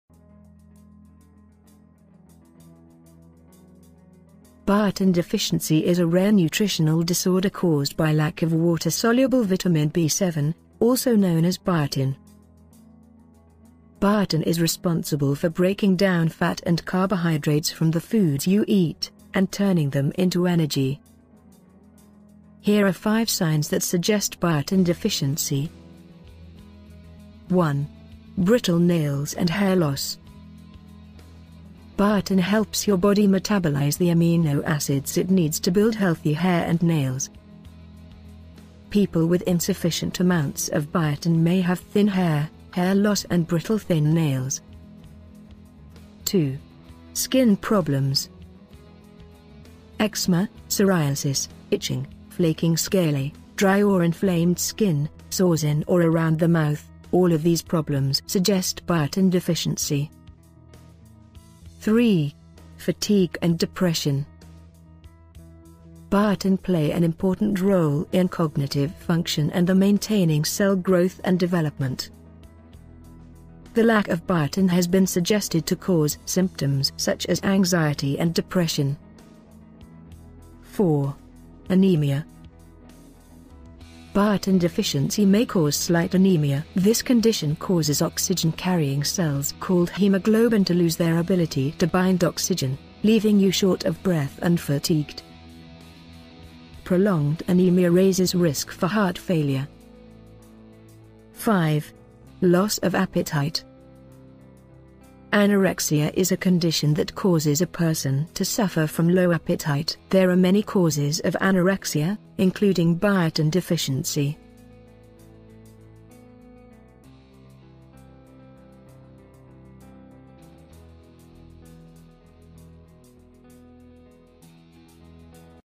5 Signs of Biotin Deficiency
Biotin deficiency is a rare nutritional disorder caused by lack of water soluble vitamin B7, also known as biotin. Biotin is one of the B complex vitamins. This group of vitamins is responsible for breaking down fat and carbohydrates from the foods you eat and turning them into energy.  Discover the signs and symptoms of biotin deficiency.

5 Biotin Rich Foods for Hair Growth